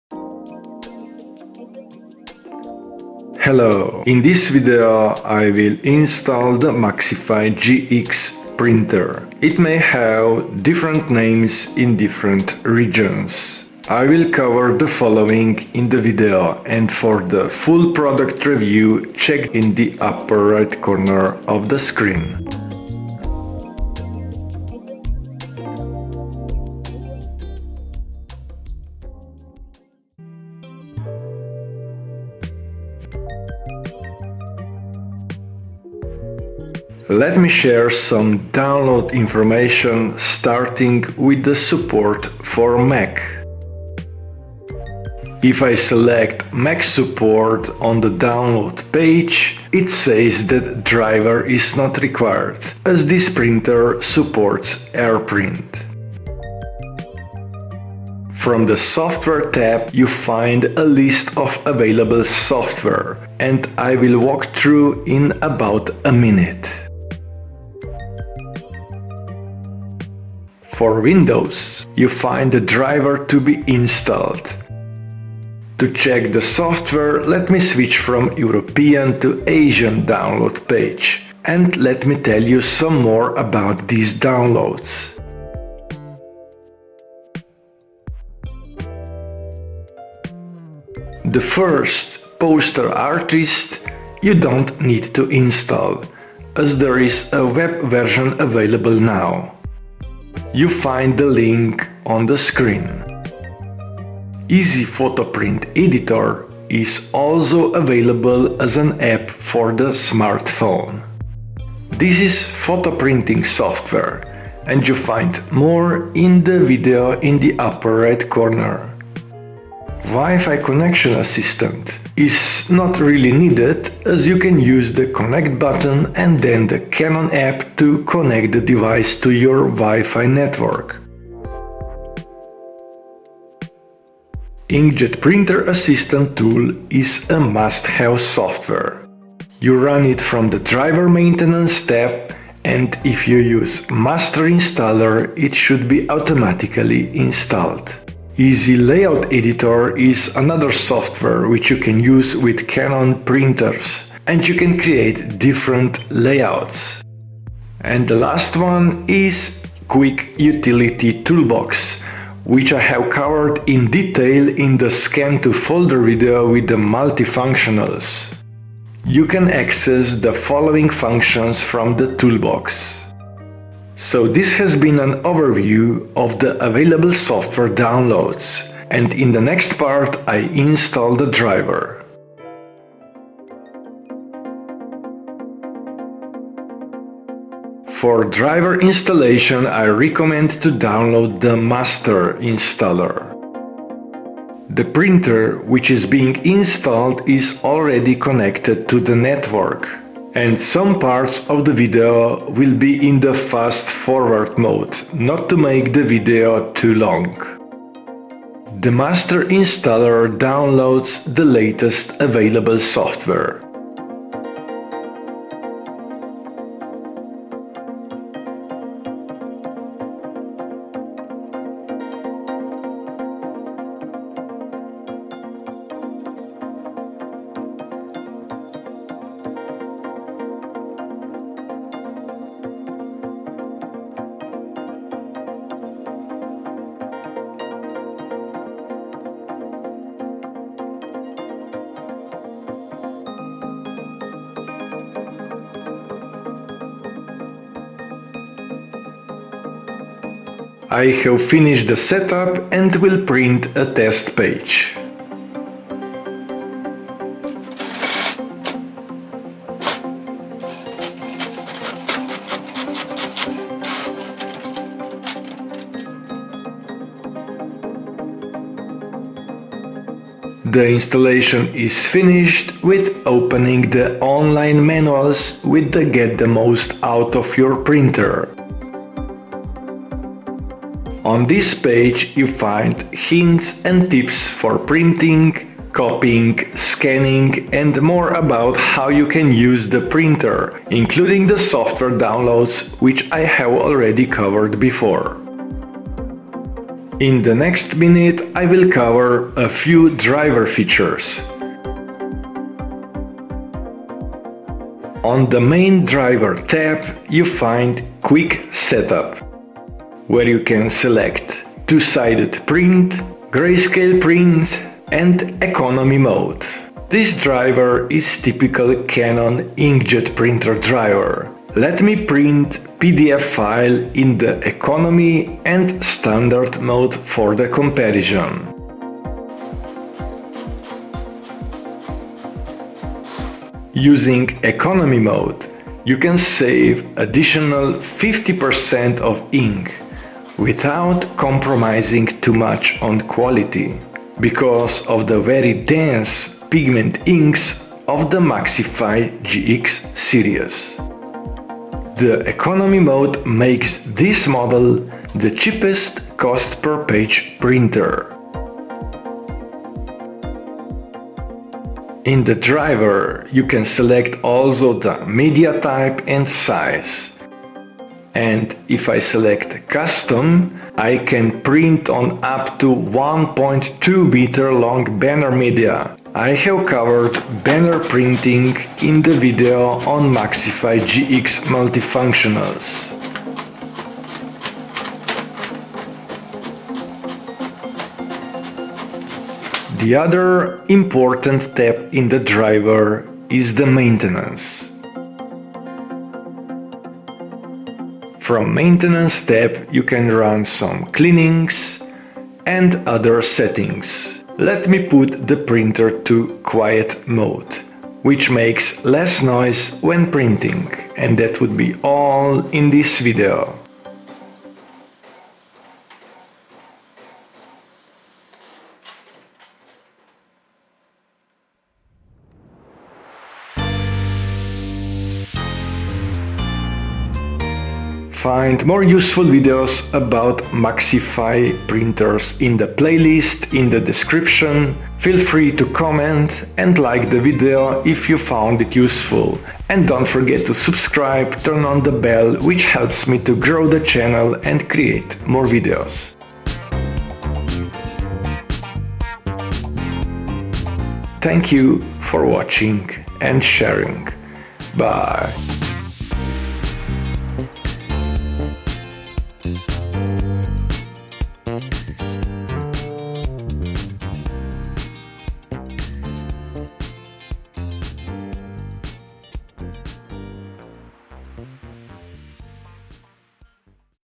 Canon MAXIFY GX5000 GX5020 GX5040 GX5050 GX5070 GX5080 GX5090 SETUP (part4) Install Driver&Software
This model is a printer only
MFPs of GX series are: GX6000 GX7000 series

MAXIFY GX5000 SETUP (part4) DRIVER & SOFTWARE
00:00 Introduction and Model Info
DOWNLOAD INFORMATION
00:30 MAC Support
01:10 Win Support
01:30 Learn about Software
         posterartist.canon
         Easy Photo Print
         WiFi Assistant Tool
         IJ Printer Assistant Tool
         Easy Layout Editor
         Quick Utility Toolbox
DRIVER INSTALLATION
03:00 Windows Driver Installation
04:30 Get the Most out of Your Printer
05:00 Learn about Printer Driver
         Economy Mode
         Media Support and Banner 1.2m print
         Maintenance Tab
         Quiet Mode
MORE ABOUT PRINTERS
07:00 More about MAXIFY GX series

Canon MAXIFY GX50X0 series
GX5010
GX5020
GX5030
GX5040
GX5050
GX5060
GX5070
GX5080
GX5090 GX5091 GX5092

ink bottles are X6 = 16, 26, 36, 46.